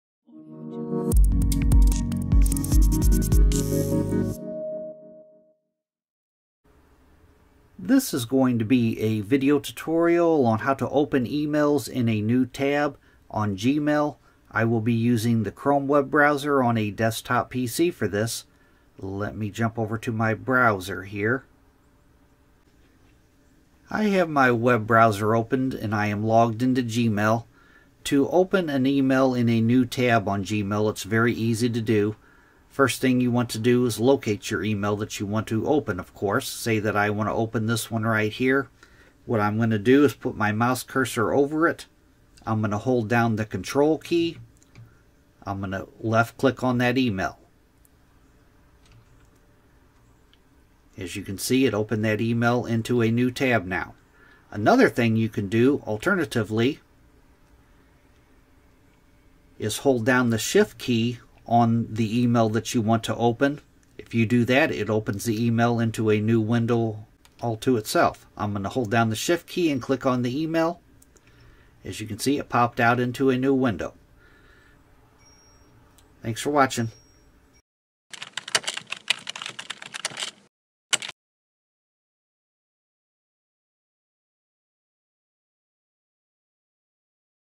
How To Open An Email Into A New Tab Using Gmail on a PC | Step-by-Step Tutorial
In this tutorial, learn how to open an email in a new tab using Gmail on a PC. Whether you want to keep your inbox organized or multitask by viewing multiple emails simultaneously, this guide will show you the simple steps to open any email in its own tab. Enhance your Gmail experience by mastering this handy feature!

Simple Steps
1. Open your browser and log into your Gmail account.
2. Place your cursor over the email that you want to send to a new tab, hold down the ctrl key on your keyboard and left click on the email.

Chapters
0:00 How To Open An Email In A New Tab In Gmail
0:21 Log Into Gmail On Your Browser
0:36 Hold Down The ctrl Key While Left Clicking On The Email
0:50 How To Open An Email In A New Window
1:00 Hold Down The Shift Key While Left Clicking On The Email